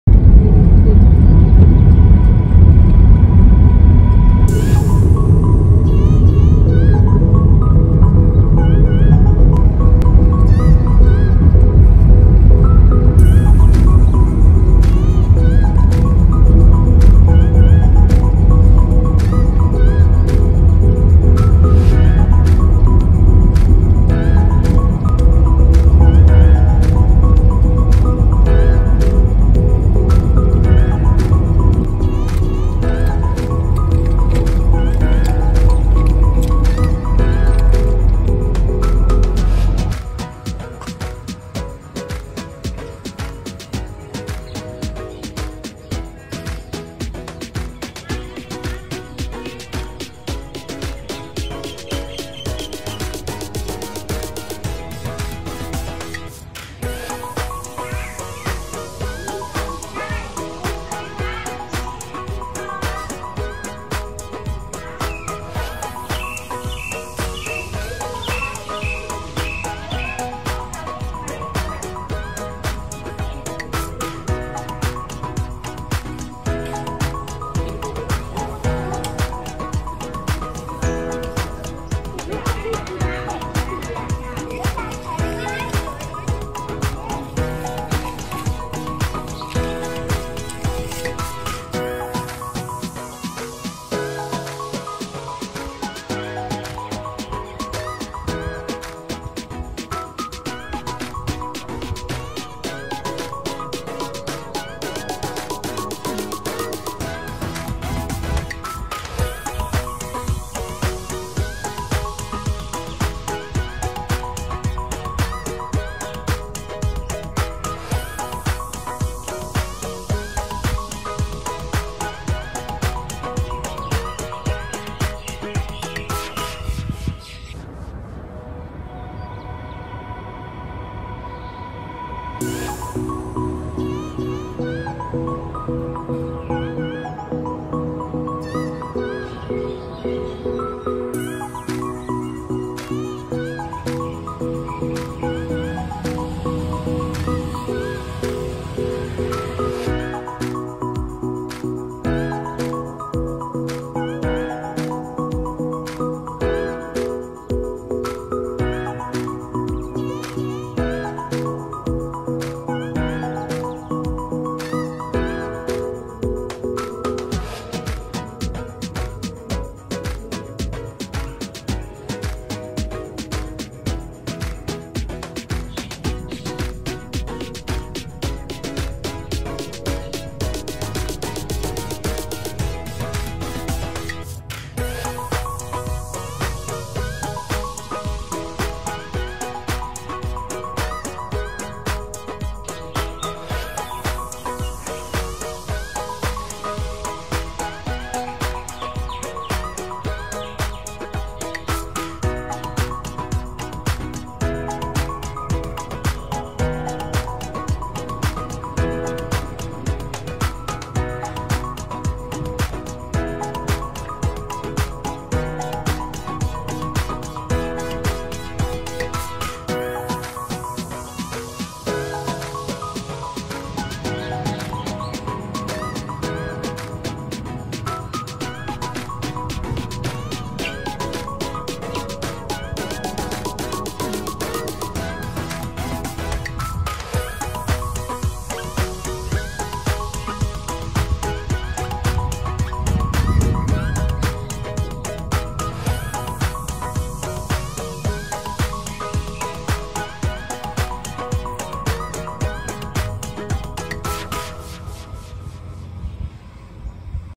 โบสถ์เก่าแก่อายุหลายร้อยปี😍วัดอุโปสถาราม จ.อุทัยธานีให้คลิปเล่าเรื่อง Apple Channel
ที่ยังคงความงดงามของโบสถ์ได้เป็นอย่างดีไม่ควรพลาดชมเด็ดขาด